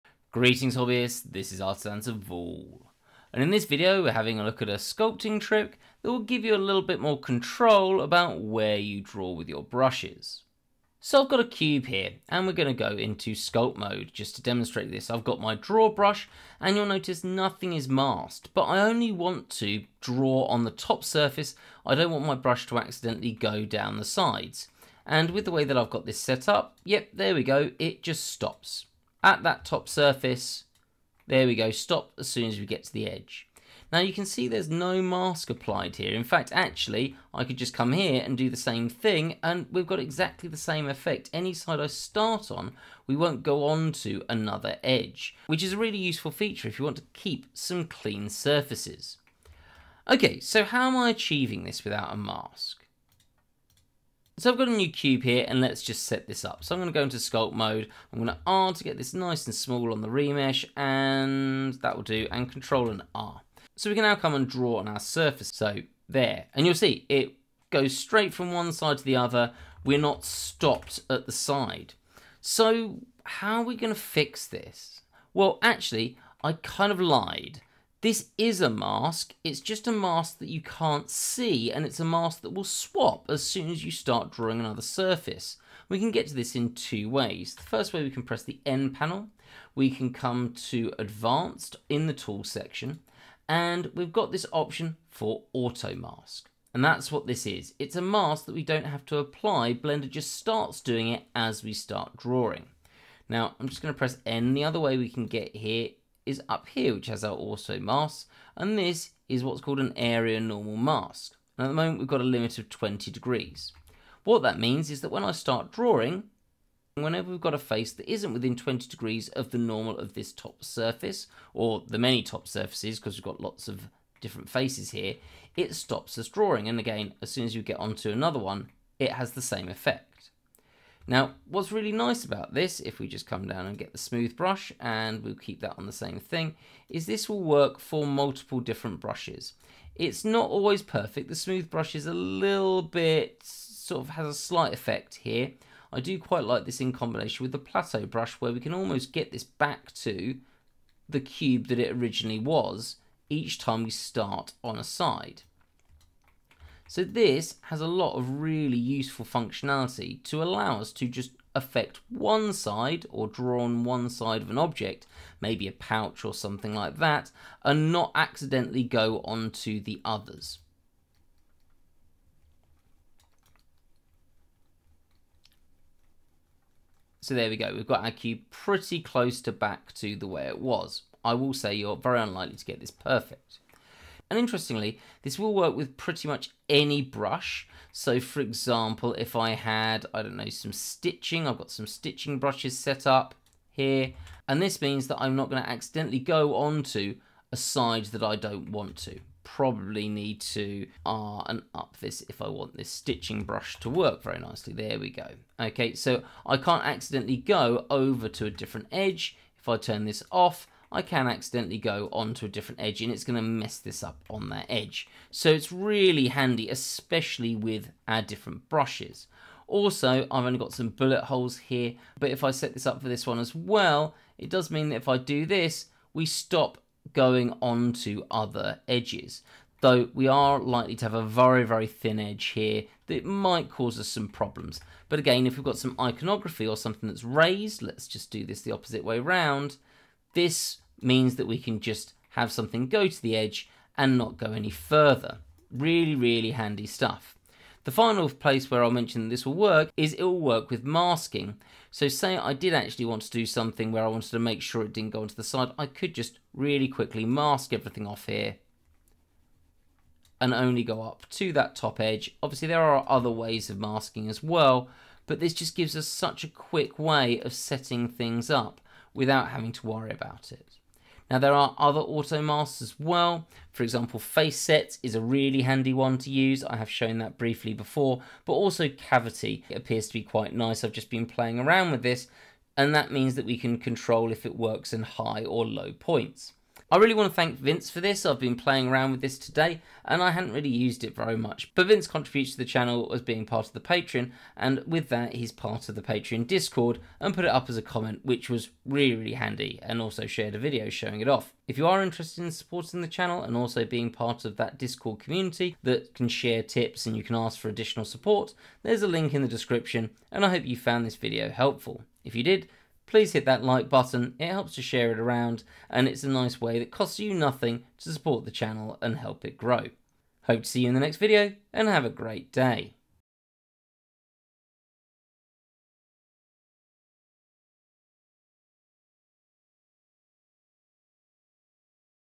Sculpting Tip for Blender: Auto-Masking
This video demonstrates a handy sculpting trick for blender that helps you control your brush, ensuring you only paint on the intended surface. With this technique, controlling your sculpting will be easier than ever.